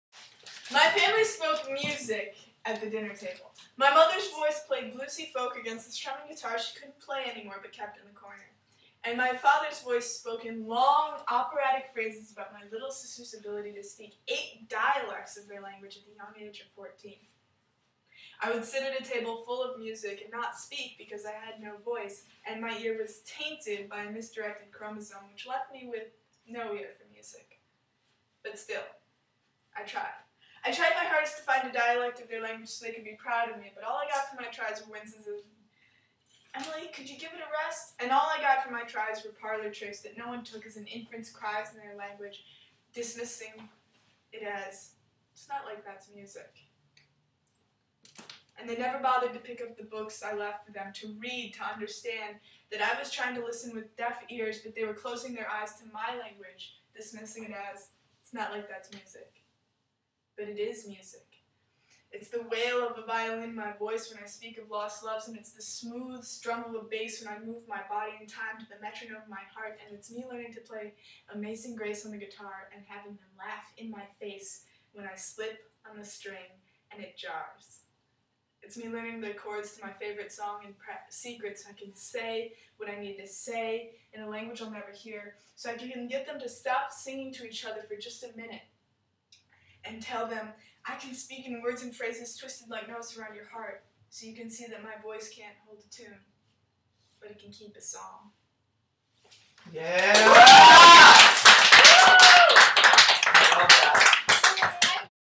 Unheard Language
An original spoken word poem by Emily at the February 2010 house show.